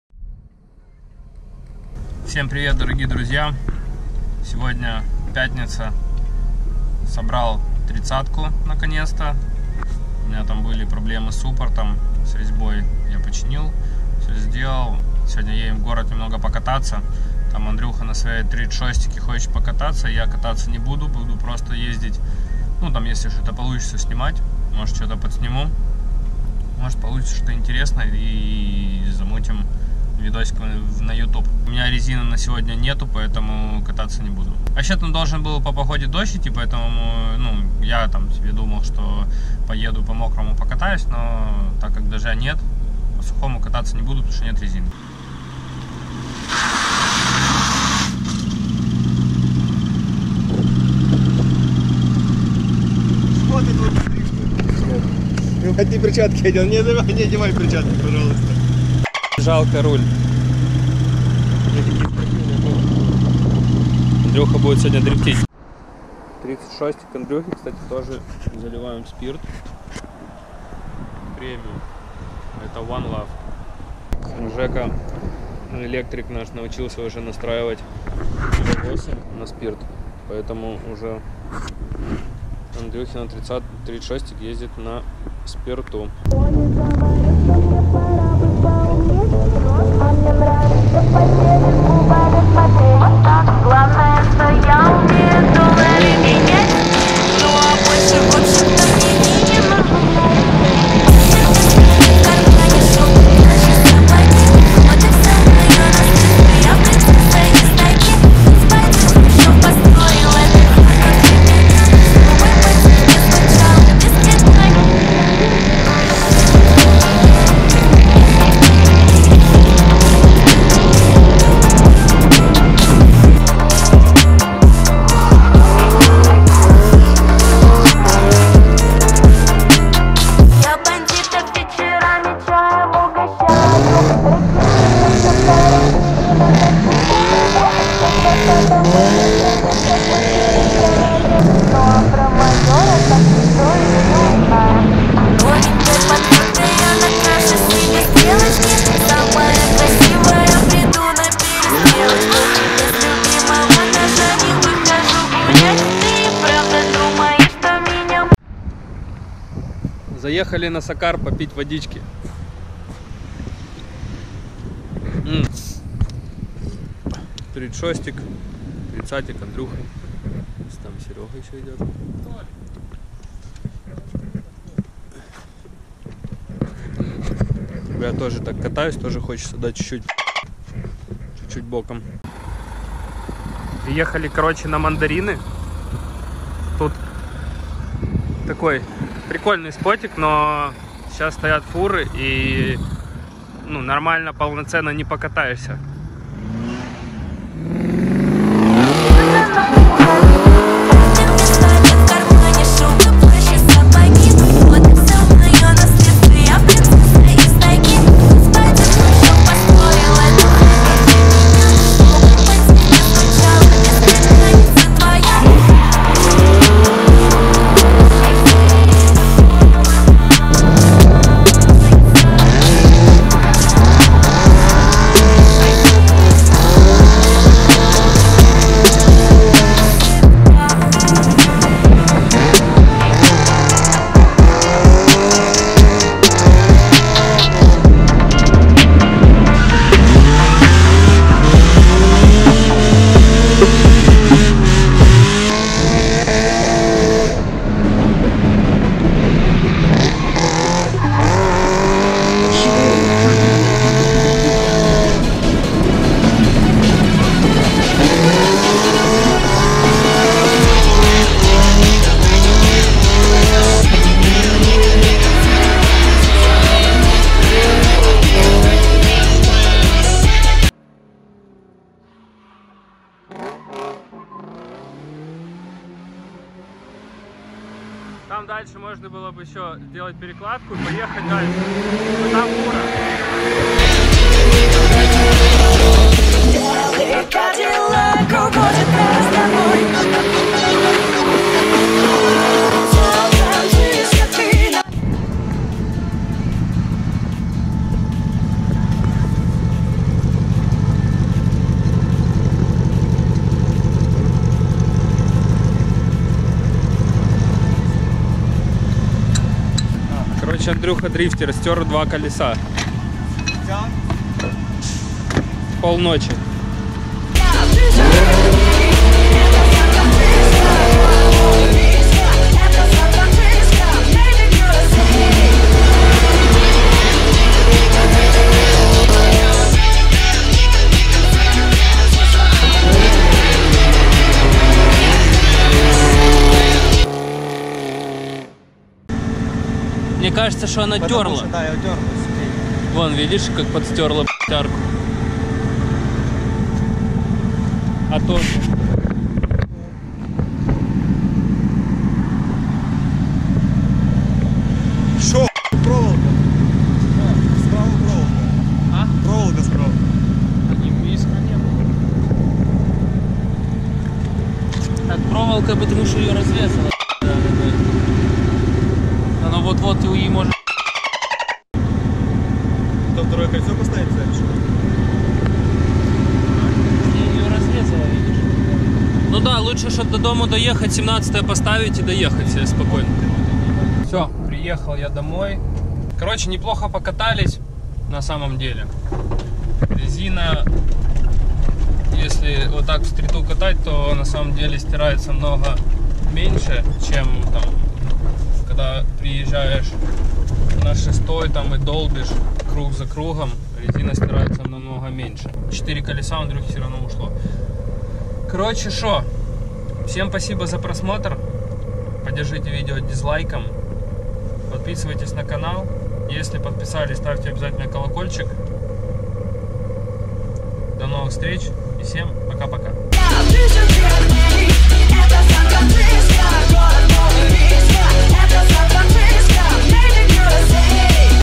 #50 УЛИЧНЫЙ ДРИФТ НА БМВ Е36 / СТРИТ ДРИФТ
Немного дрифта на сейвах, пока прикатывается к тачке